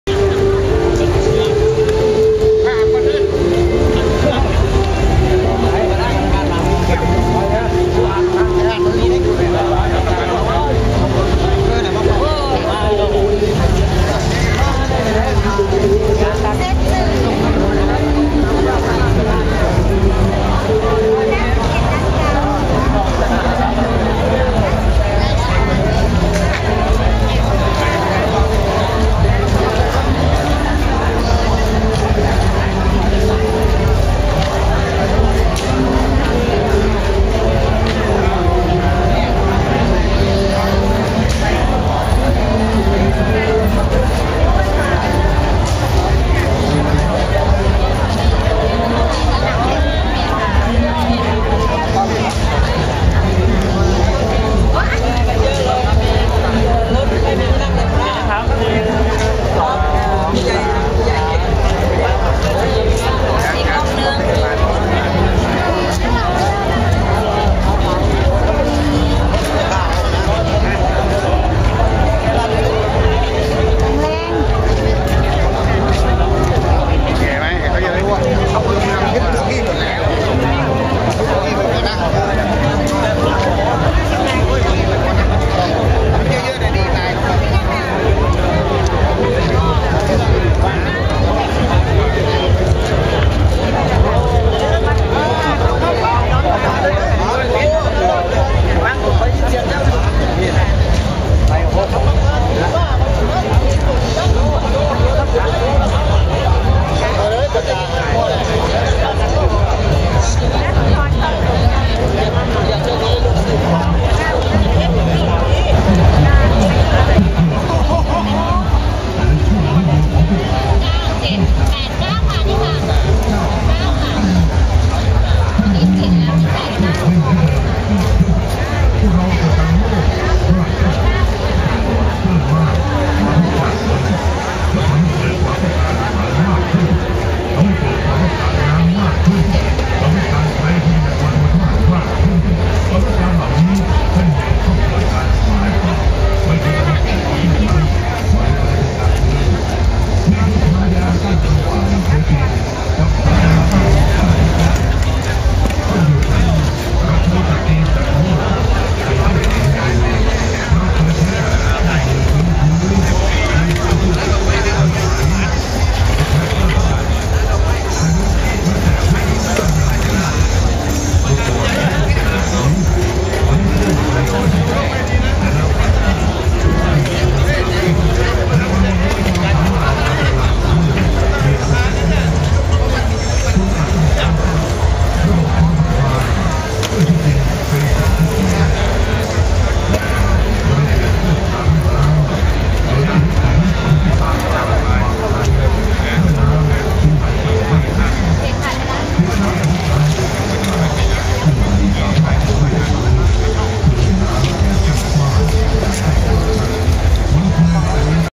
ก่อนพิธีเปิดงานประเพณีไหลเรือไฟนครพนม 2554 ศาลากลางจังหวัดนครพนม
บรรยากาศก่่อนพิธีเปิดงาน  "  ประเพณีไหลเรือไฟ ออกพรรษา และ กาชาด จังหวัดนครพนม  ประจำปี พ.ศ  2554   ที่ ลานหน้า  ศาลากลางจังหวัดนครพนม  วันที่  5  ตุลาคม  2554